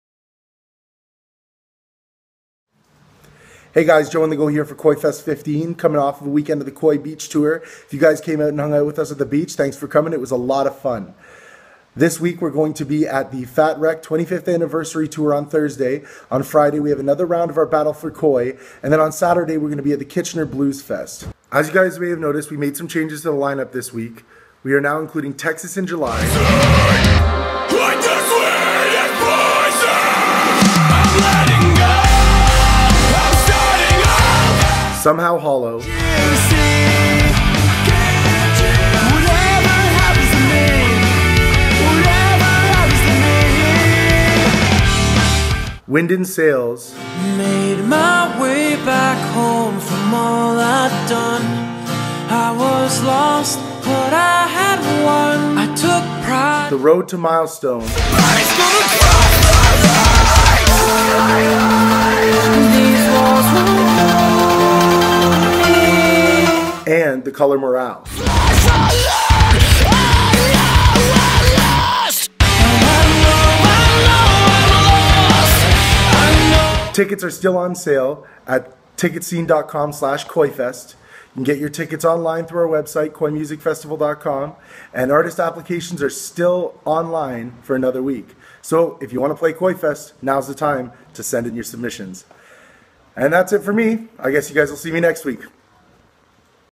KOIFEST 2015 Weekly Update No. 9 - Fat Wreck, Bluesfest, Battle For KOI!
JoeOnTheGo filling your lives with KOIFest15 tidings!Find out where we will be this weekend: Fat Wrecked, BluesFest, and oh BABY it is BATTLE TIME! Check it outttttttt!